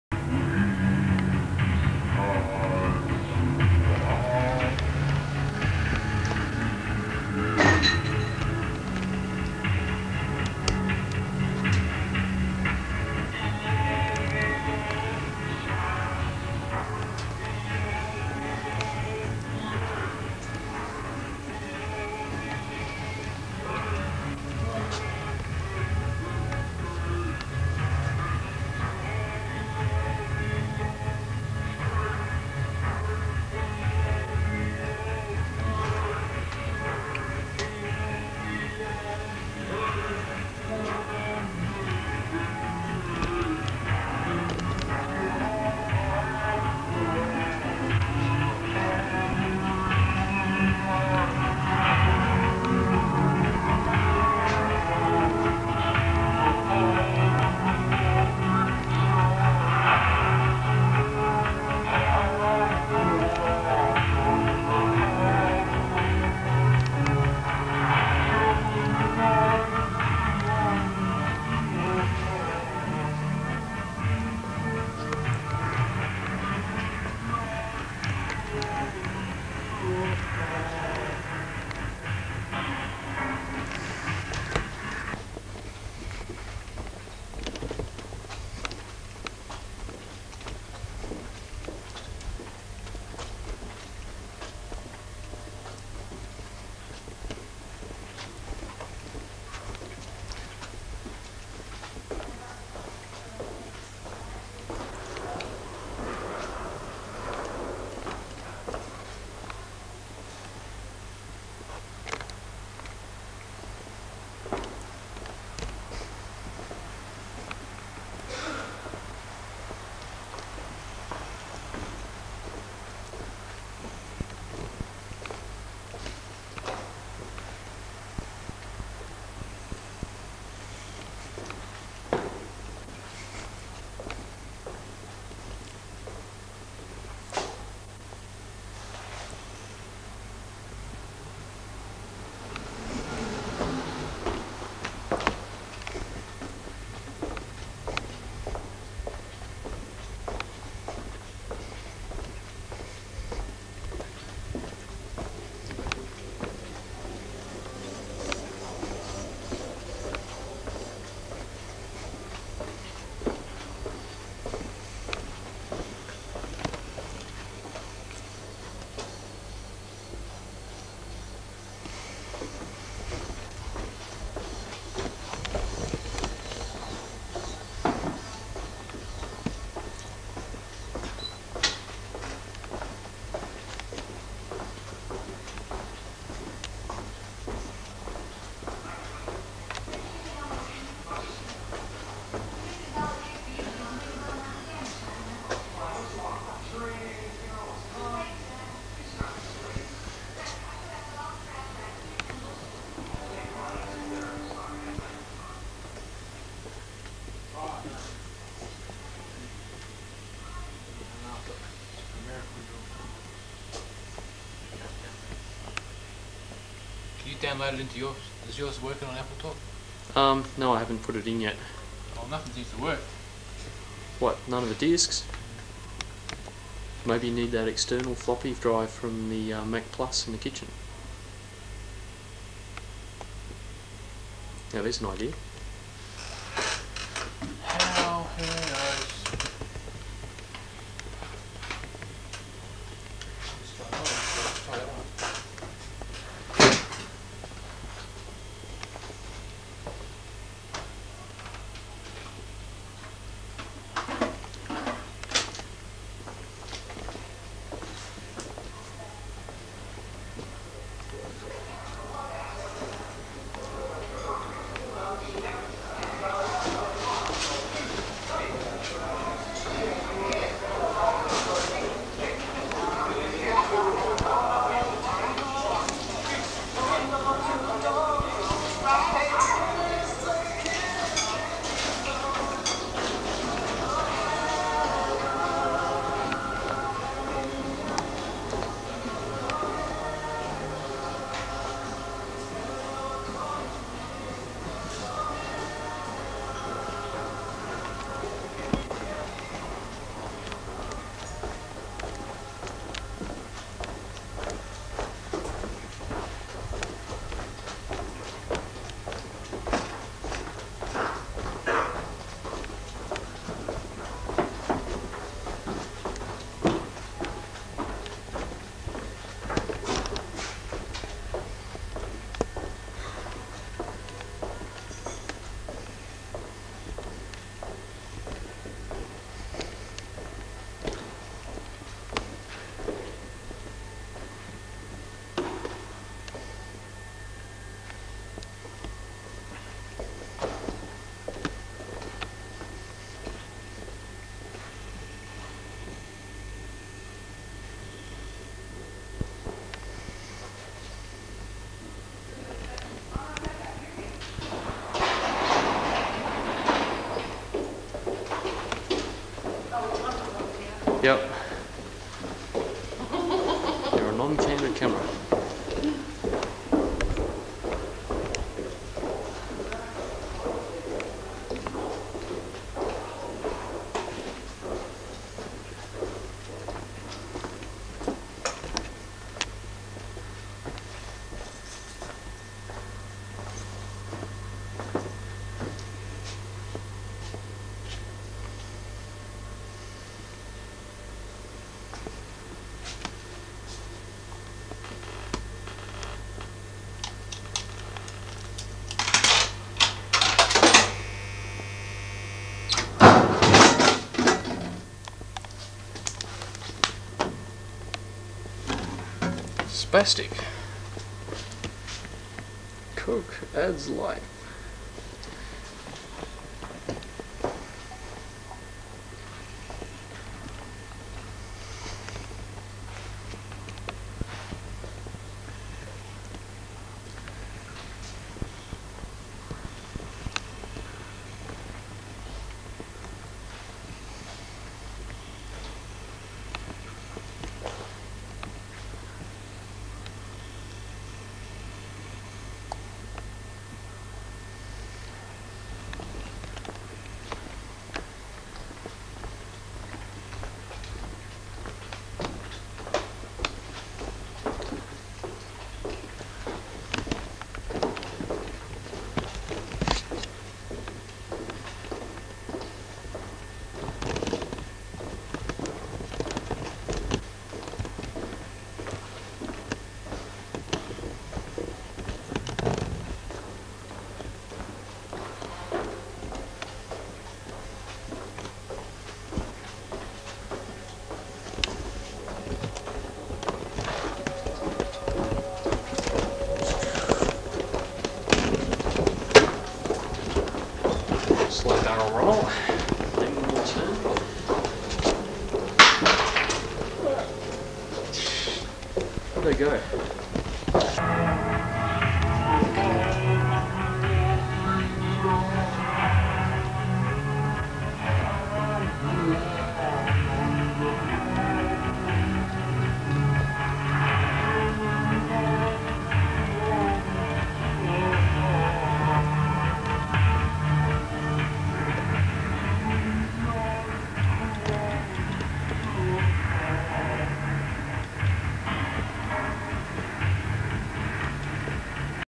Cyberspace_warehouse_night
Cyberspace Warehouse, Glebe. Walking from my studio, on the corner above the old Harold Park hotel, through the warehouse to Sam Shovels studio, where my brother Martin is helping Sam with some software. Then out to the common room, and back through the art gallery to the loading dock. Then back to the studio where Martin is petting Mozart my black cat.

Matthew Spong